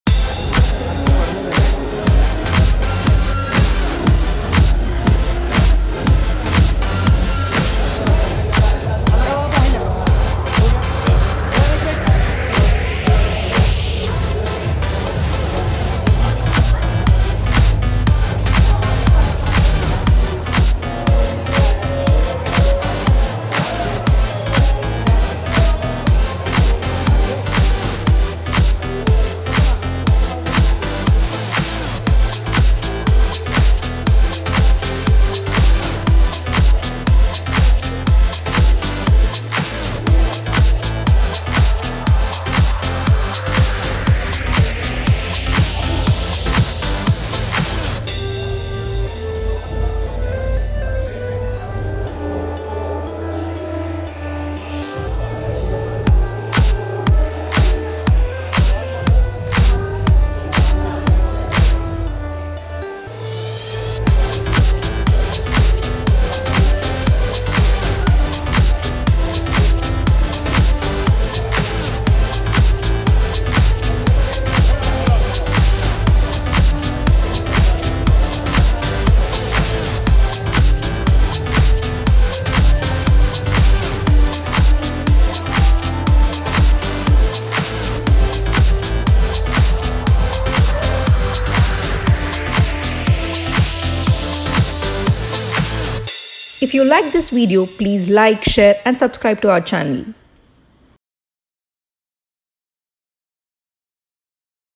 Lords Of The Drinks Asia's Longest Bar launch in Mumbai | Asianet Newsable
Bollywood celebs like Poonam Pandey, Divya Agarwal, Navin Agrawal, and others were spotted at Lords Of The Drinks Asia's Longest Bar launch in Mumbai.